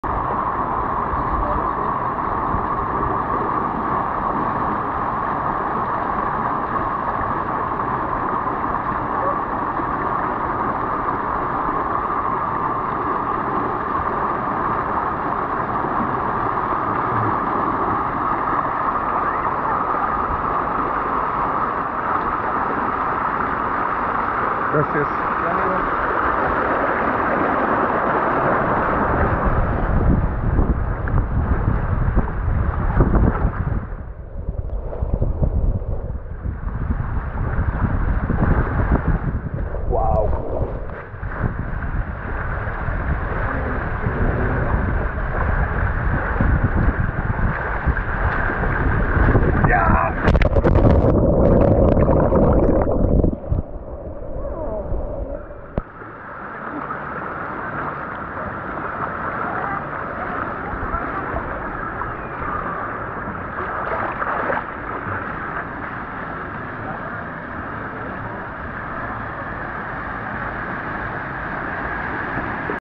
Body Water Slide at El Rollo Parque Acuático
Body Waterslide Ride at El Rollo Parque Acuático water park in Tlaquiltenango, Morelos, Mexico